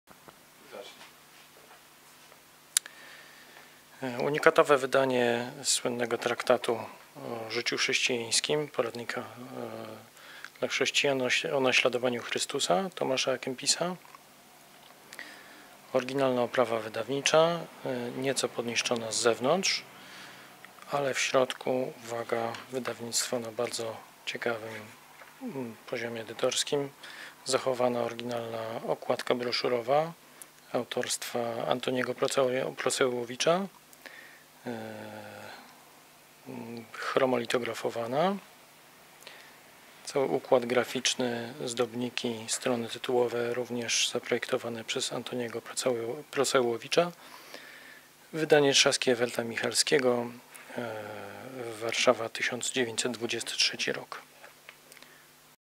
Tomasz A KEMPIS "De imitatione Christi. O naśladowaniu Chrystusa ksiąg cztery" (1923)
"De imitatione Christi. O naśladowaniu Chrystusa ksiąg cztery" à Kempisa (ok. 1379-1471), mistyka, wydanie książki ważnej  dla rozwoju katolickiej duchowości i rozwoju religijności indywidualnej u kresu średniowiecza.
Projekt okładki, stron tytułowych i zdobienia Antoni Procajłowicz.
Wydanie z 1923 r., nakładem Księgarni Trzaski, Everta i Michalskiego, okładka broszurowa w litografii, ozdobne inicjały w tekście, zdobione karty tytułowe. Oryginalna oprawa wydawnicza z zachowaną dekoracyjną okładką broszurową.